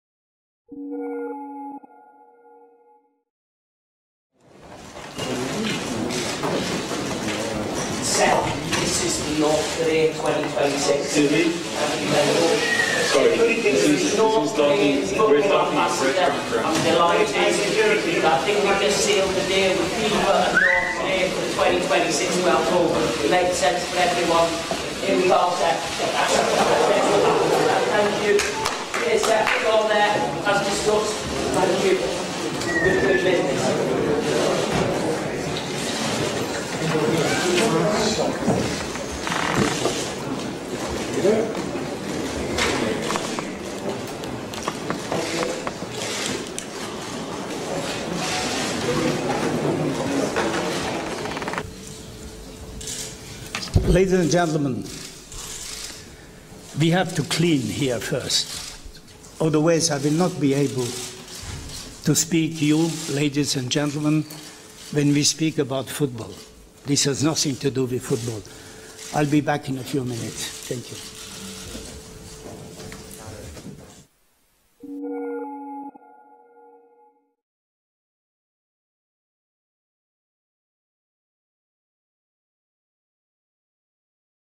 FIFA's Blatter pranked with fake money shower
A British comedian known as Simon Brodkinthrows banknotes at FIFA PresidentSeppBlatter as he arrives for a news conference after the Extraordinary FIFA Executive Committee Meeting at the FIFA headquarters in Zurich, Switzerland. (Photo: Reuters)

This video was originally published on 21 July 2015